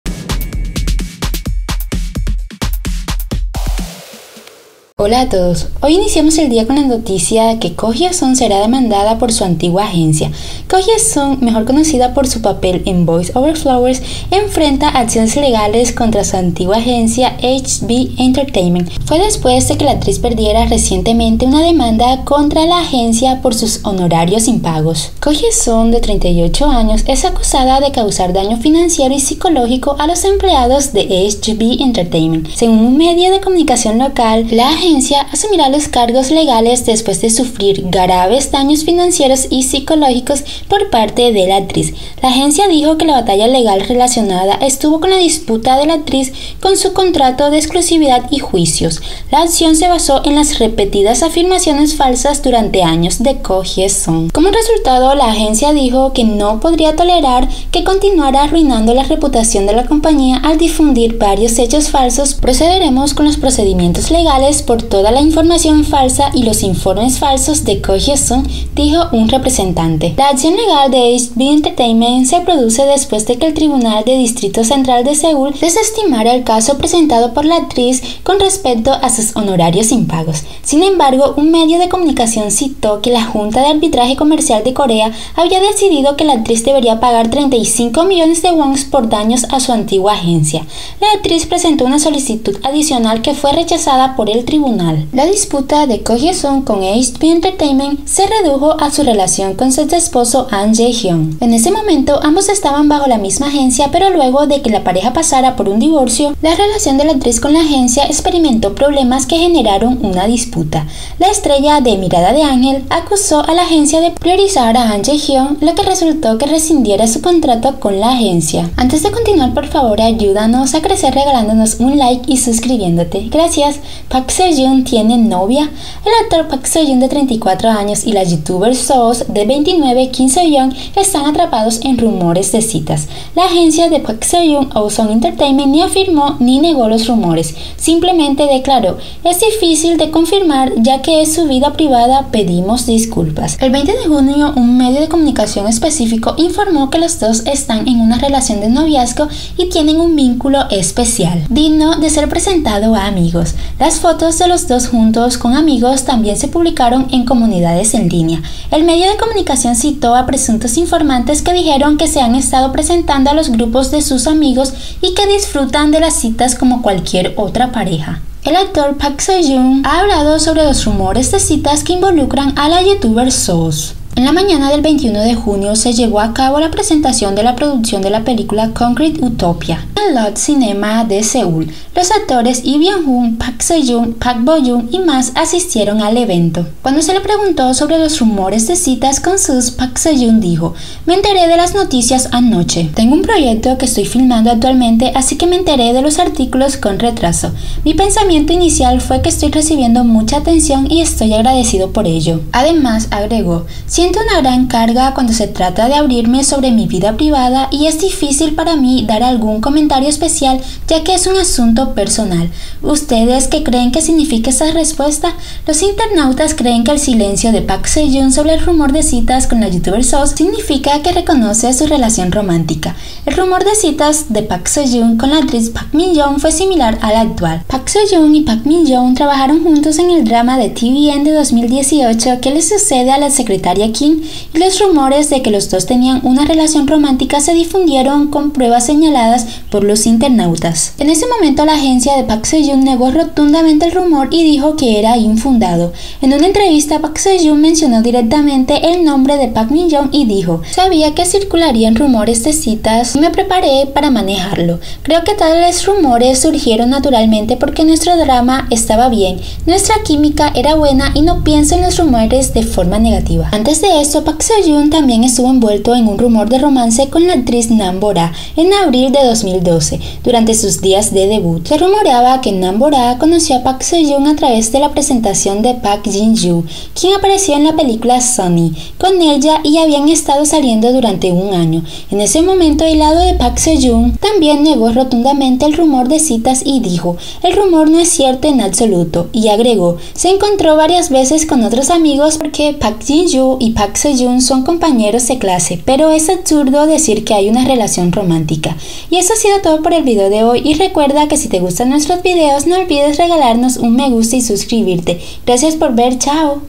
PARK SEO JOON CONFIRMÓ SU NOVIAZGO ¿? GO HYE SUN DEMANDADA POR DAÑO PSICOLÓGICO Y FINANCIERO.
En el video de hoy te traemos las últimas noticias del mundo del espectáculo
el mundo del "entretenimiento coreano no deja de sorprendernos y esta semana no es la excepción por eso en el video de hoy te traemos las últimas noticias que están dando mucho de qué hablar.